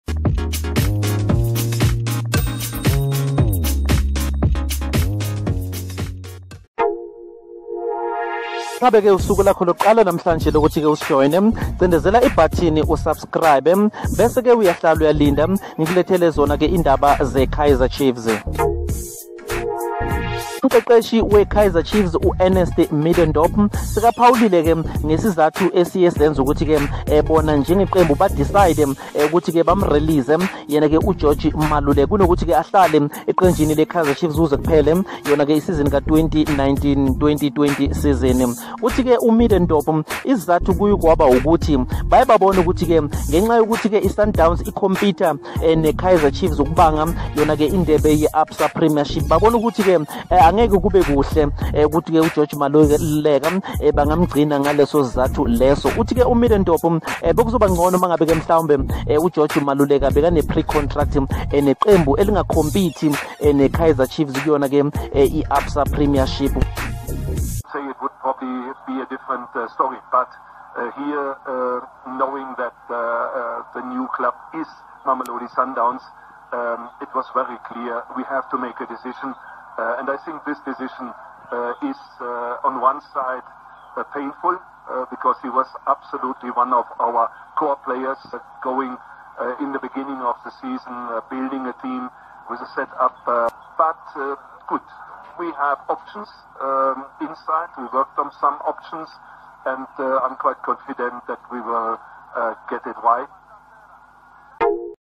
Kaizer Chiefs Coach - On George Maluleka & starting Lineup. pslnews amakhosi4life. ( 1-1)
naturena based side coach explained the reasons that lead them to get rid of George Maluleka to join sundowns .